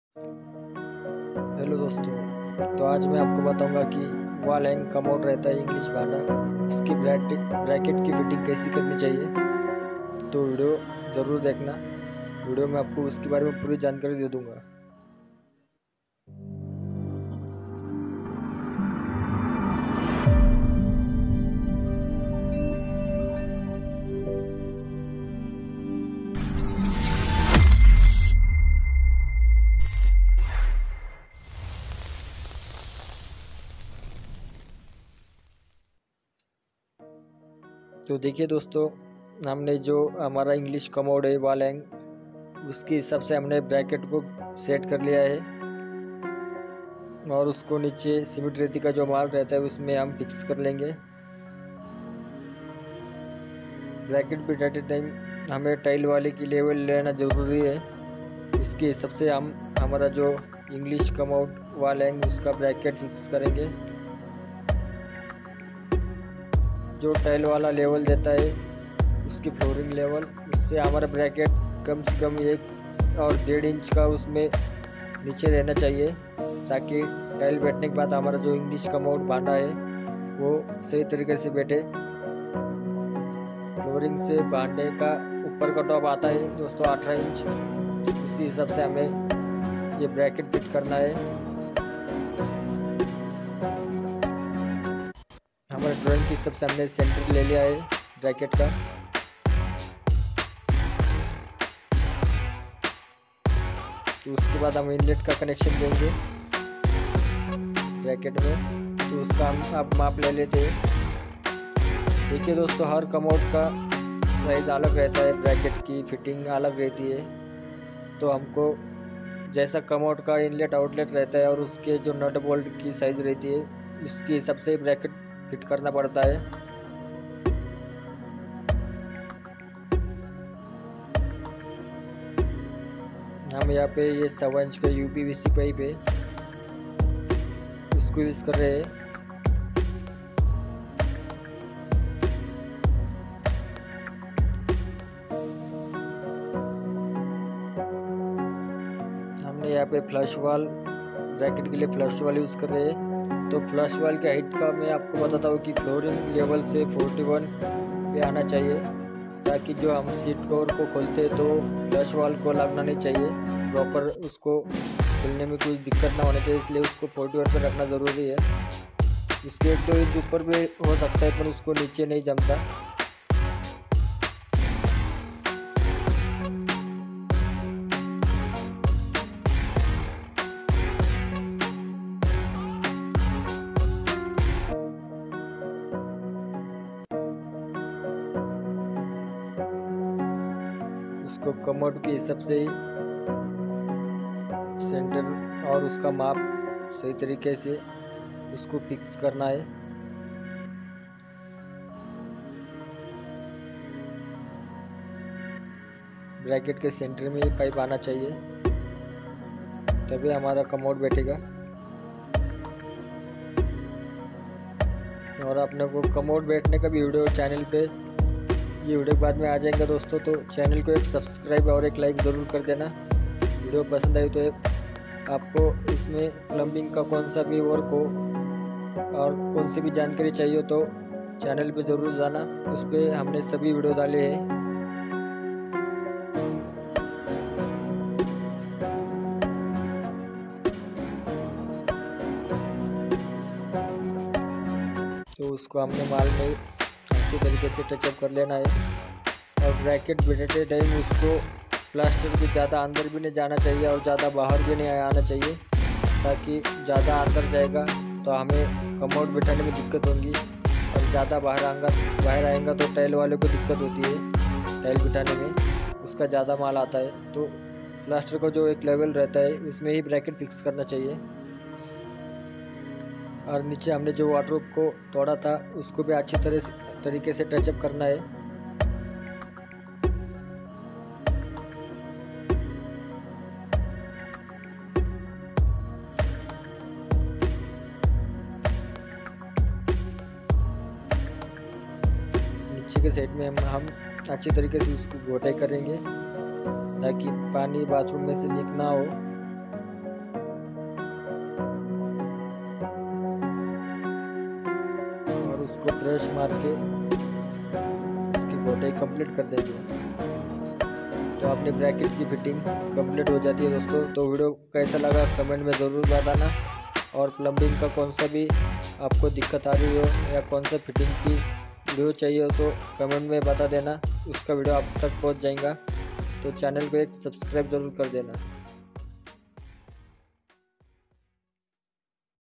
दिवार के अंदर MM में ब्रेकेट Fix करें / Wall hack conceal bracket fitting@thesevensjartgallery6270
नमस्कार दोस्तों आज हम आपको इस वीडियो में वॉल हैंग कमोड चेयर के बारे में पूरी जानकारी डिटेल के साथ में देने वाले हैं तो आपका पैर इस वीडियो को जरूर देखें और चैनल को शेयर करके सब्सक्राइब जरूर कर दे थैंक्स धन्यवाद

 thanks 😊😊😊